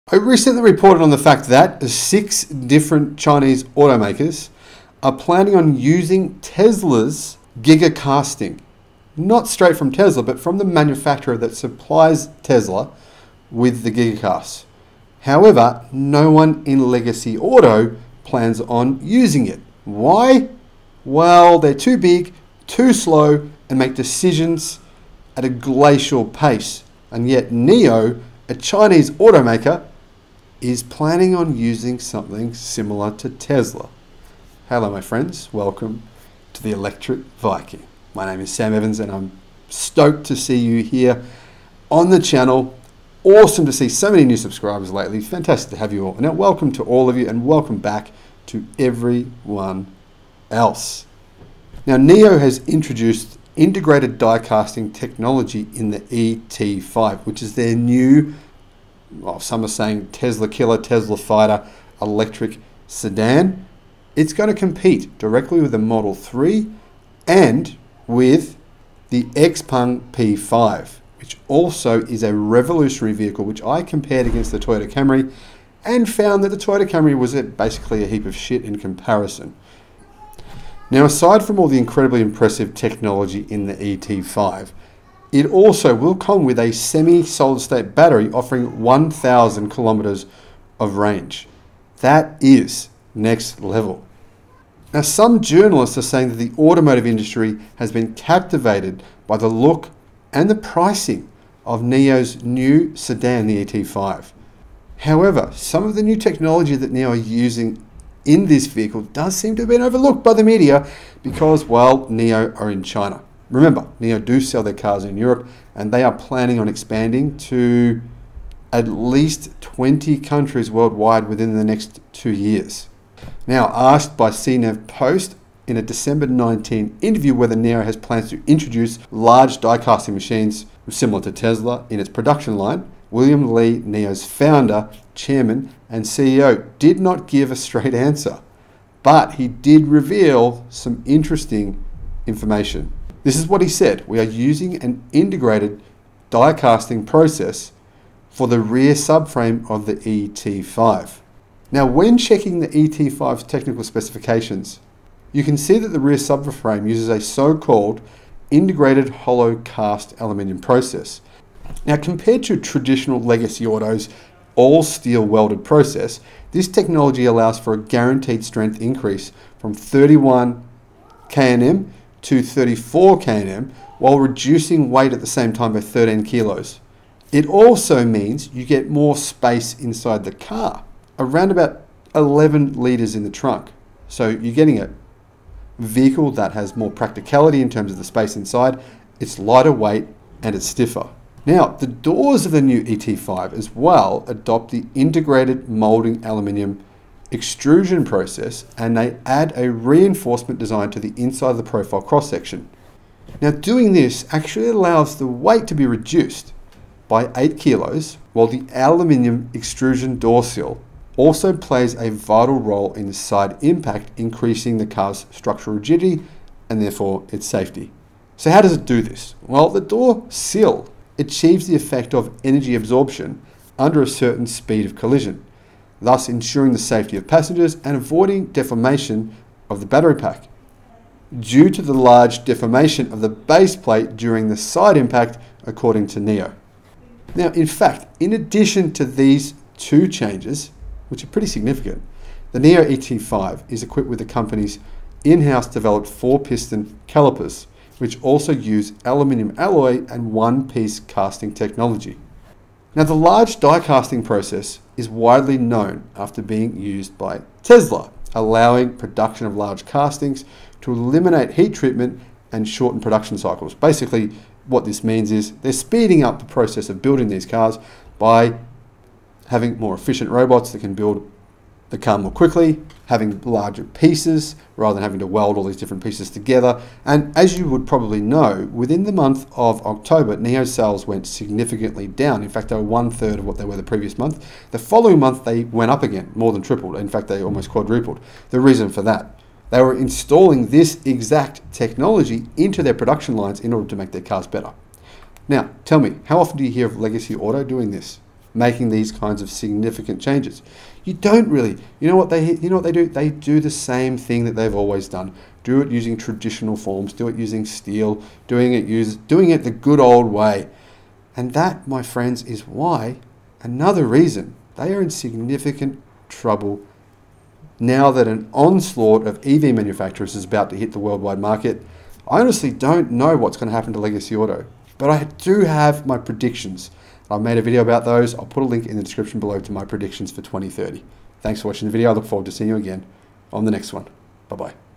NIO using NEW manufacturing techniques similar to Tesla
China EV war: Tesla demand fuels ‘home run’ quarter as NIO, Xpeng step up rivalry with five-fold surge in deliveries | South China Morning Post